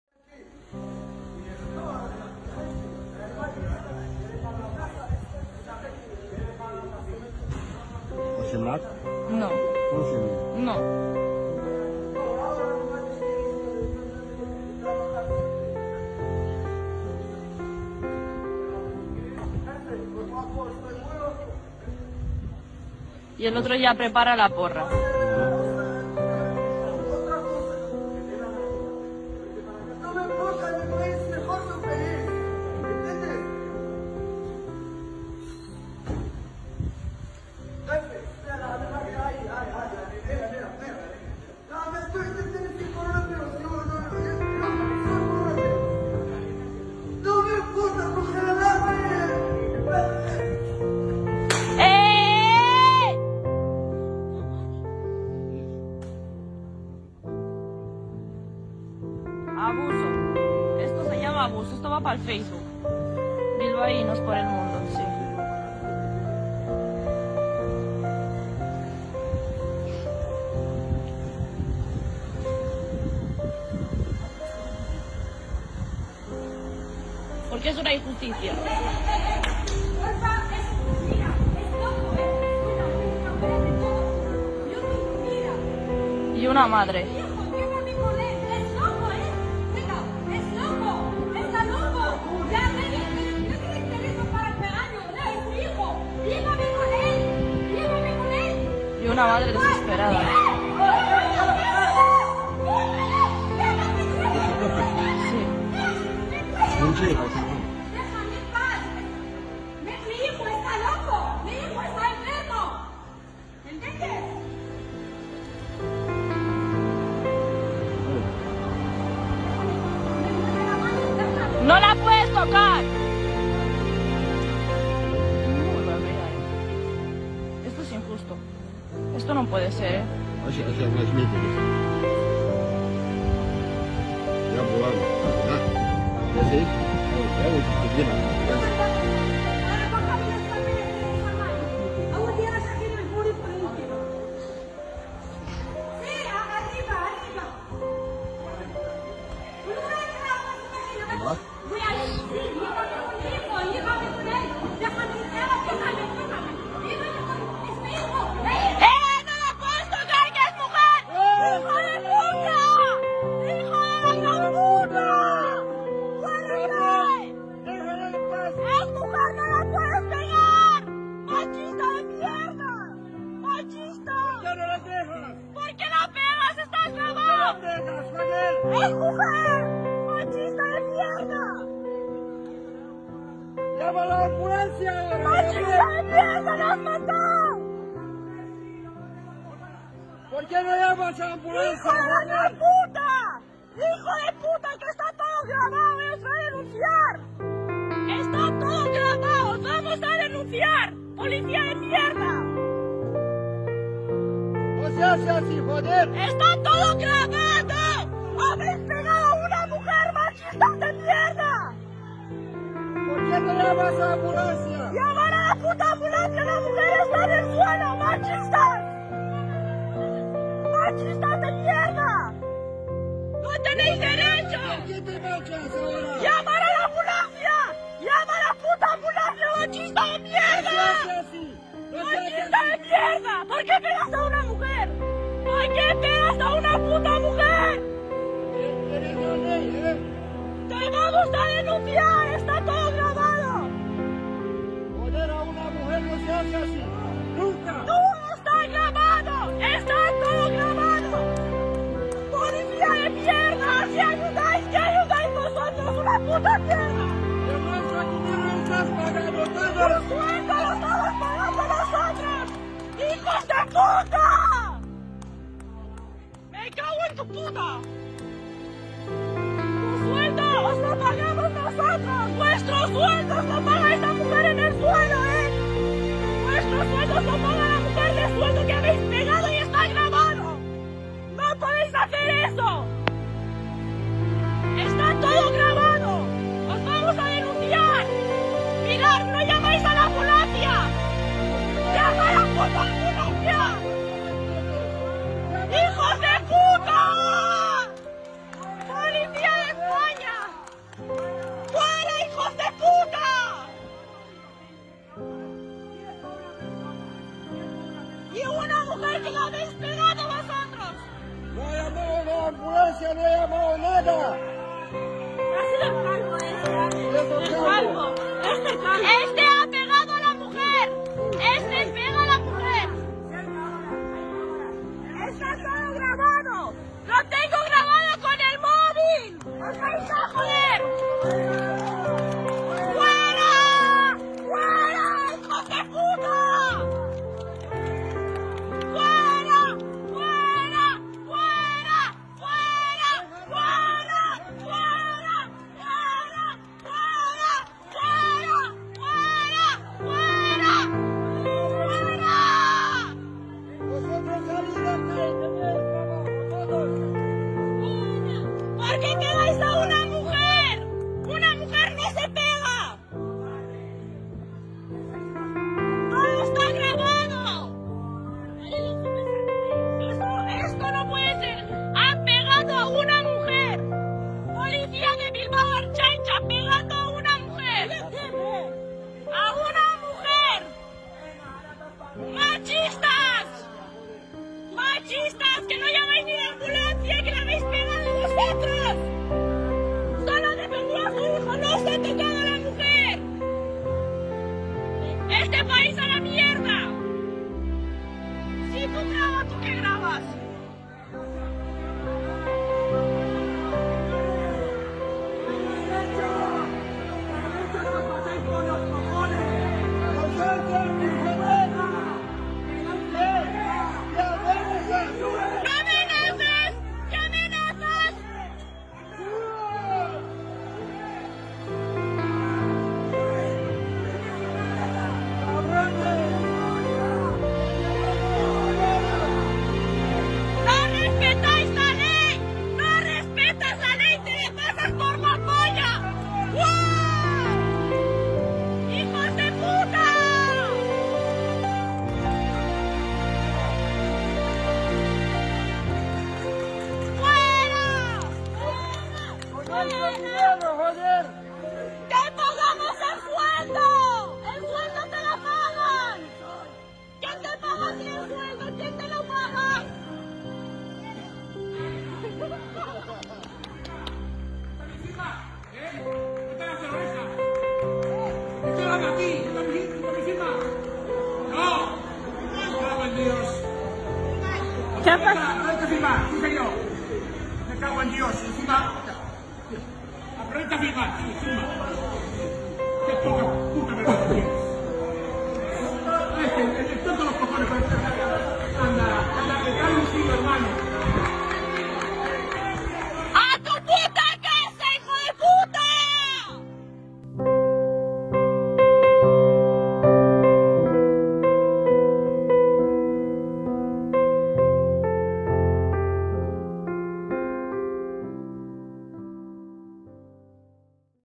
POLICE ATION AGAINST LOCKDOWN BREAKOUT // SPAIN MARCH 2020
This footage was recorded somewhen at the end of march somewhere in Spain. What's going on in people's minds while watching this? How long does it take for people to say it's enough?